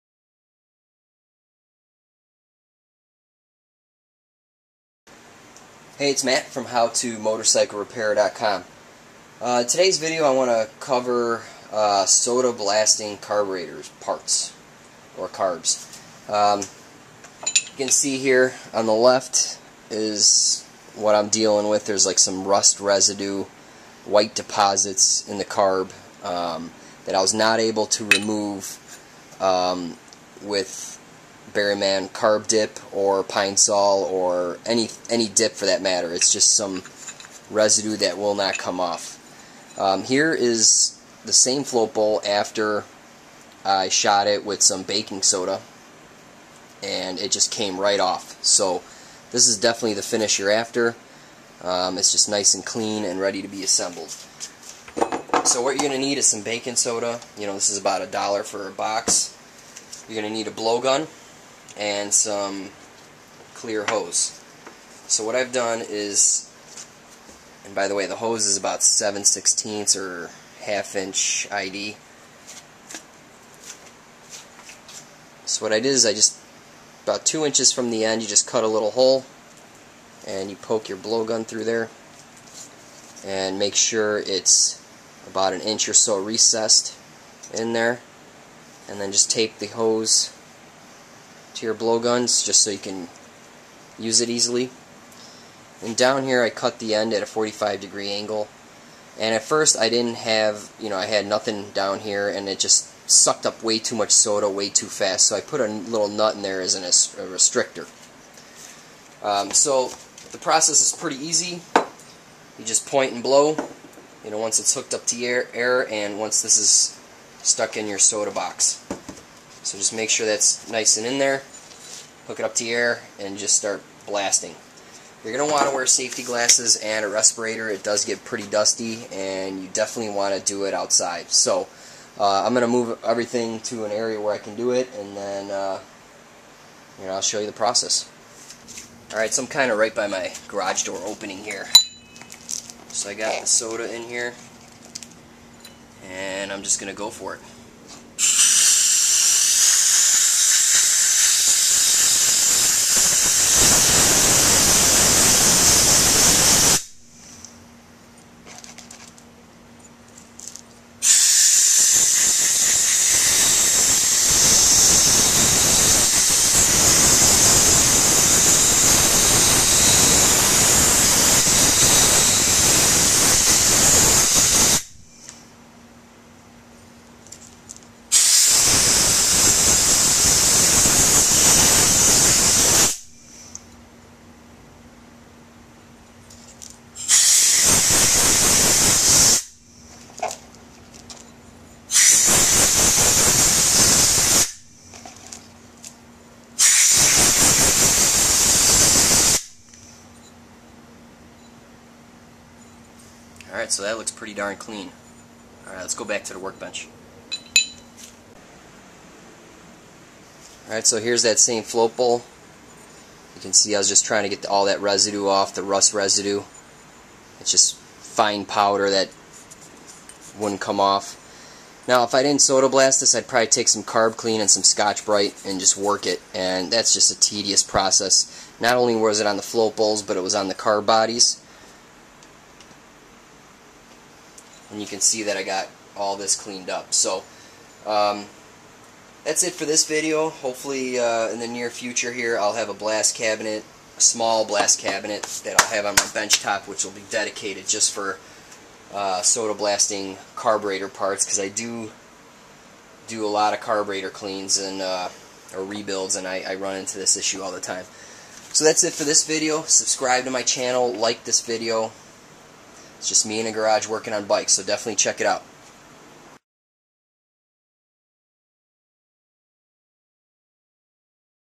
How-To: DIY Soda Blaster - Cleaning Motorcycle Carburetor
Learn How-To Repair and Maintain your own Motorcycle!